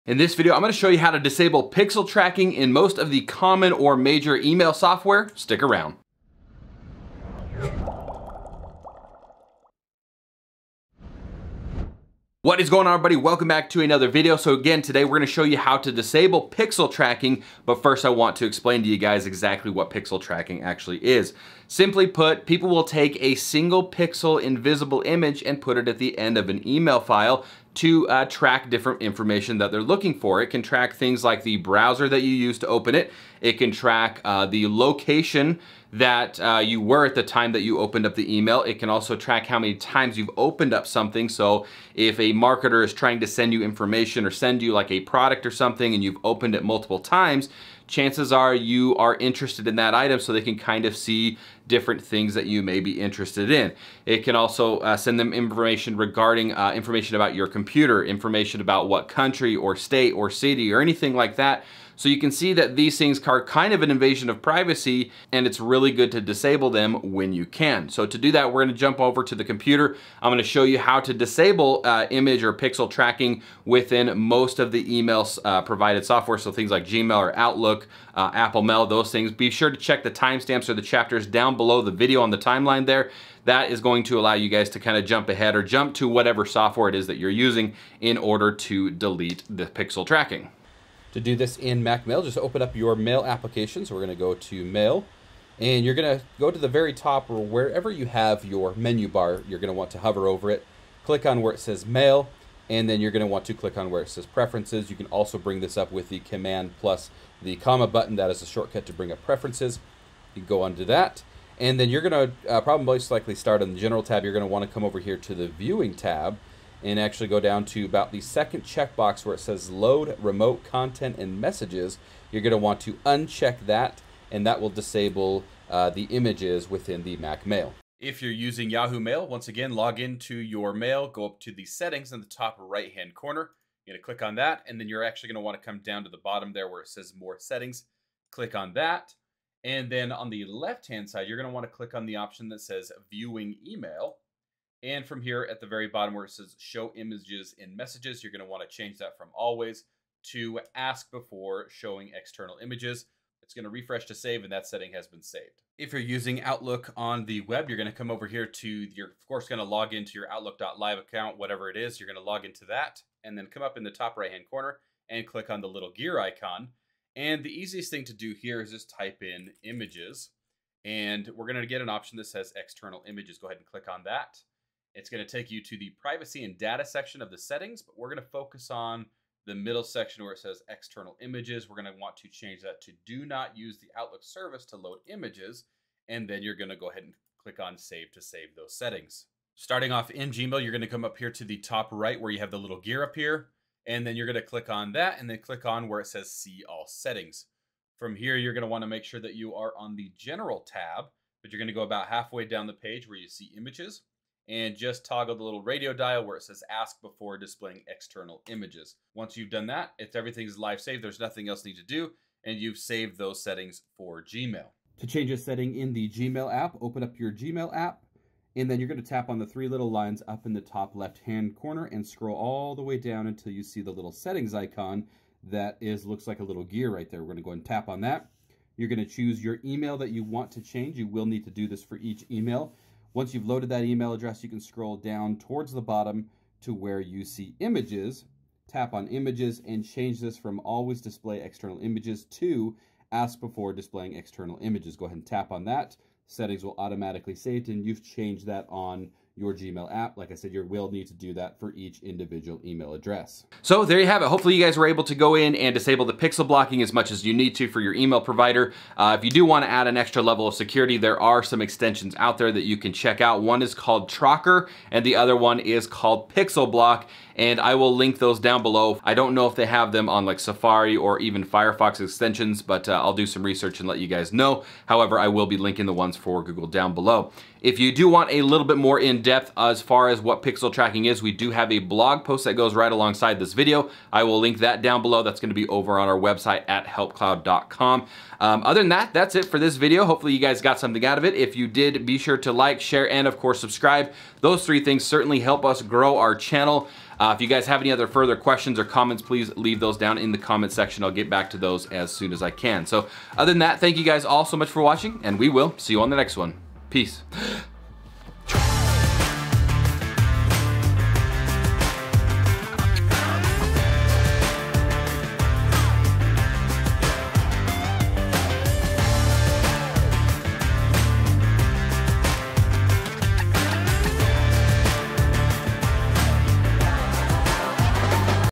What is Pixel Tracking & How to Disable It (Gmail, Apple Mail, Outlook, and Yahoo! Mail)
Simply put, pixel tracking is when a person adds an invisible single pixel image to the end of an email which can track when the email is opened, where it is opened, etc. This image contains a line of code which allows marketers and other nefarious ne’er do wells to track your information.
If you have images enabled on your email program, then when you receive an email with a tracking pixel embedded in it, a request is sent to the sender’s server for the image, and this then sends a whole host of information to the sender.  In this video, we show you how to disable pixel tracking and block automatic downloads within most major email softwares (Gmail, Apple Mail, Yahoo! Mail, and Outlook).
HelpCloud blog post How to Block Tracking Pixels and Automatic Downloads Within All Major Email Providers

Extensions
Trocker:

0:00 Intro
0:16 What is Pixel Tracking
1:20 Apple Mail
2:05 Yahoo Mail
2:30 Outlook
3:10 Gmail
3:34 Gmail App
4:13 Final Thoughts